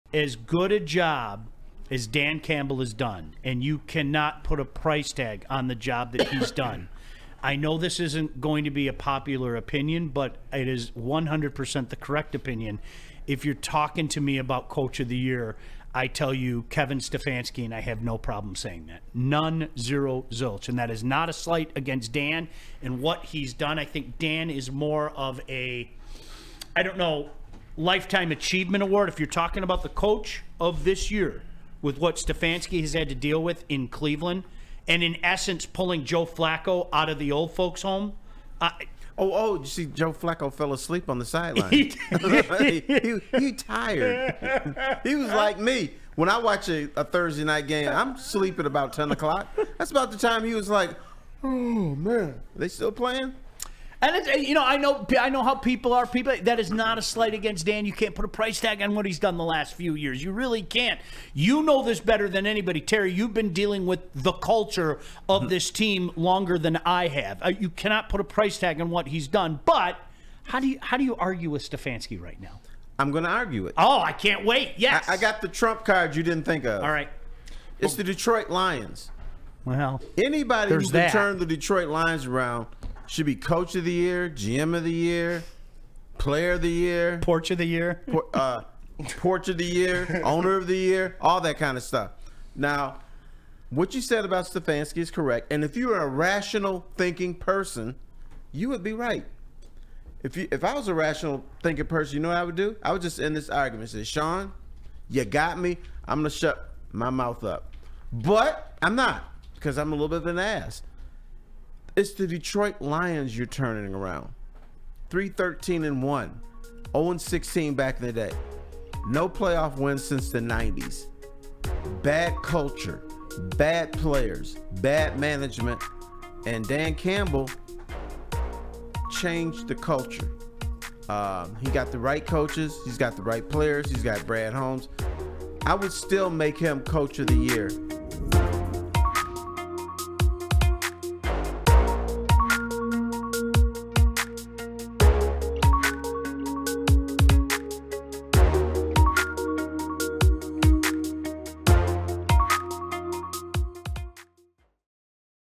Dan Campbell Has a CHANCE To Win, Detroit Lions
Sean Baligian and Terry Foster discuss if Dan Campbell has done enough to win coach of the year.